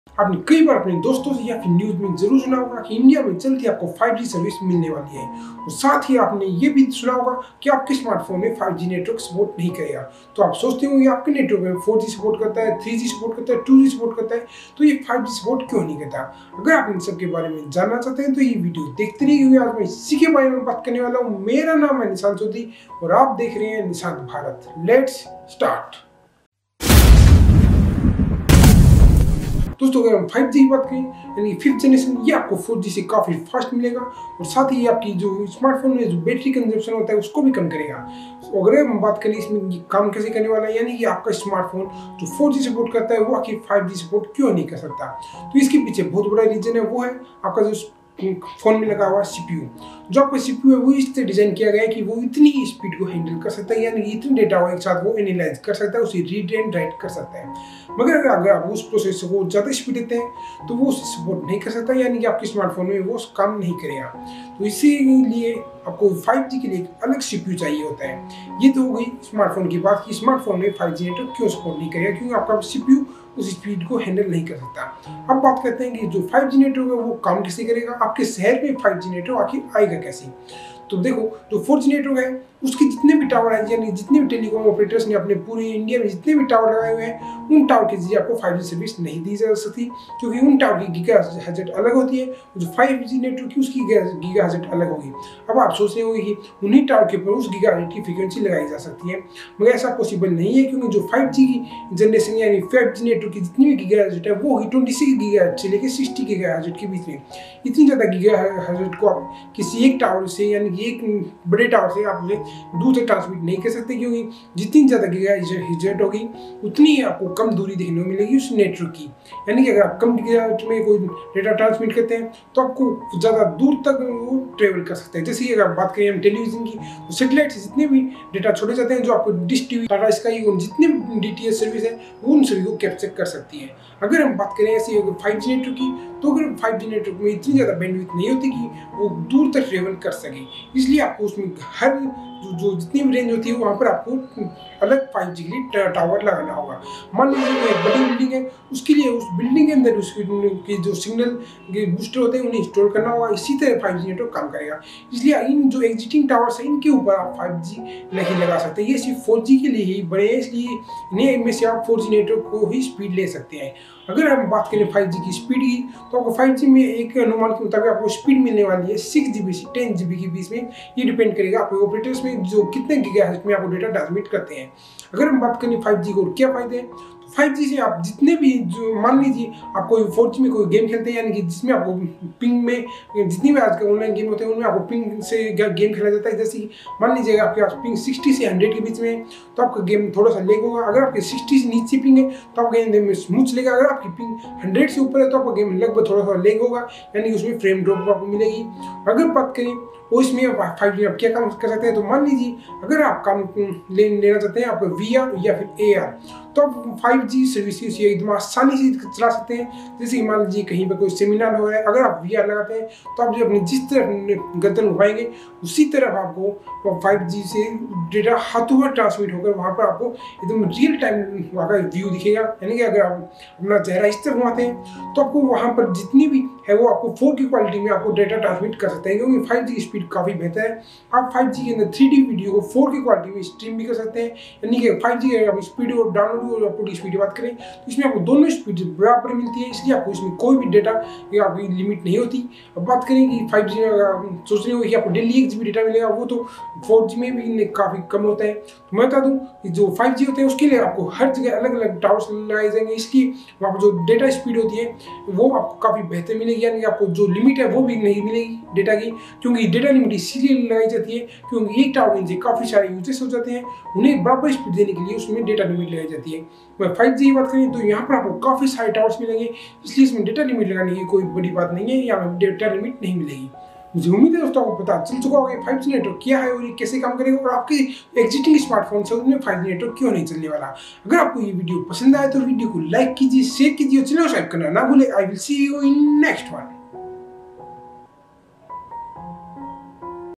How 5G Works | Why Present Smartphones Not support 5G network? | Explained | NishantBharat
5G is the fifth generation of cellular mobile communications. It succeeds the 4G (LTE/WiMax), 3G (UMTS) and 2G (GSM) systems. 5G performance targets high data rate, reduced latency, energy saving, cost reduction, higher system capacity, and massive device connectivity. The first phase of 5G specifications in Release-15 will be completed by March 2019, to accommodate the early commercial deployment. The second phase in Release-16 is due completed by March 2020, for submission to the ITU as a candidate of IMT-2020 technology.[1]

The ITU IMT-2020 specification demand for speeds up to 20 gigabits per second, achievable with millimeter waves of 15 gigahertz and higher frequency.[citation needed] 3GPP is going to submit 5G NR (New Radio) as its 5G communication standard proposal. 5G New Radio can include lower frequencies, from 600 MHz to 6 GHz. However, the speeds in these lower frequencies are only slightly higher than new 4G systems, estimated at 15% to 50% faster.[2]